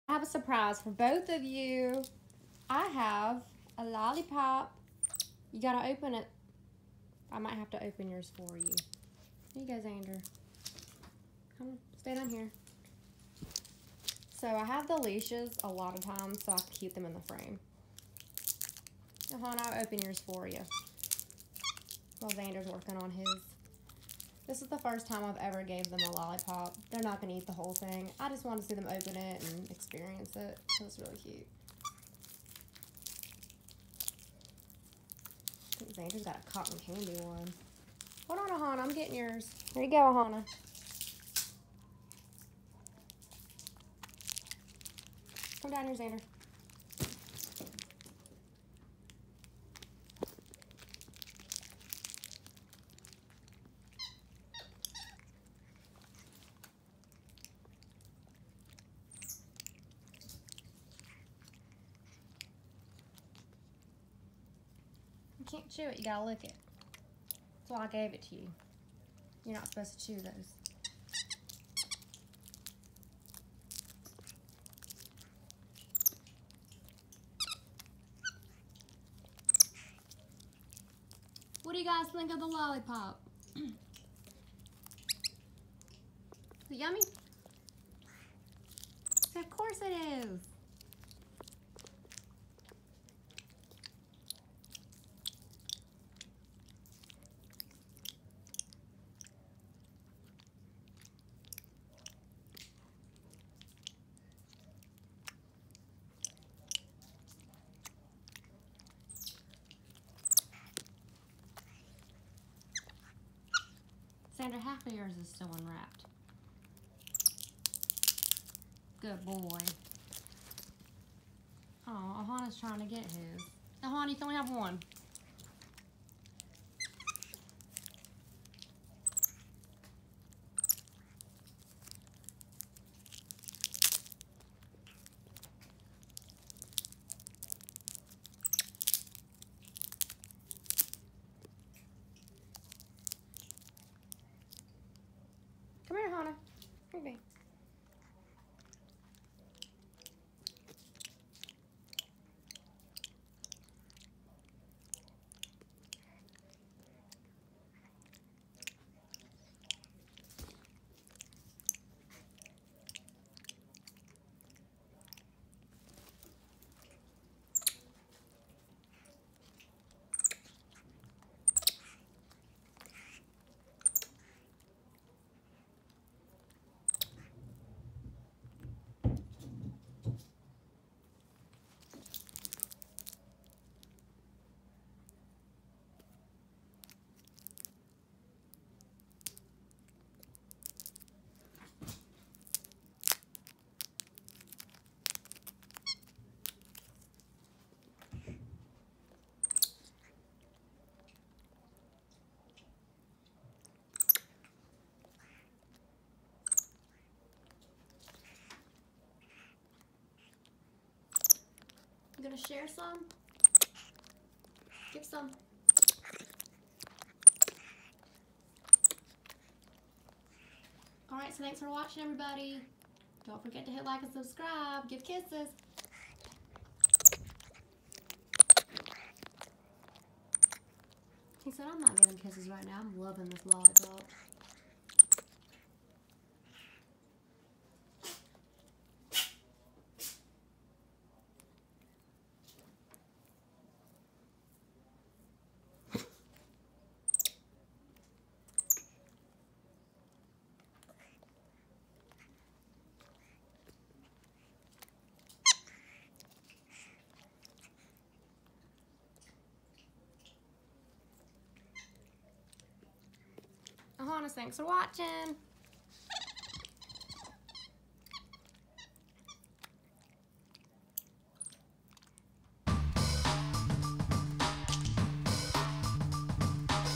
Capuchin Monkeys LOVE LolliPOPS!
Capuchin MonkeyXander and his brother Ohana enjoy a lollypop for the first time. Watch how smart MonkeyXander is opening up the lolli and holding it. These boys are super intelligent. Enjoy!

Gift ideas :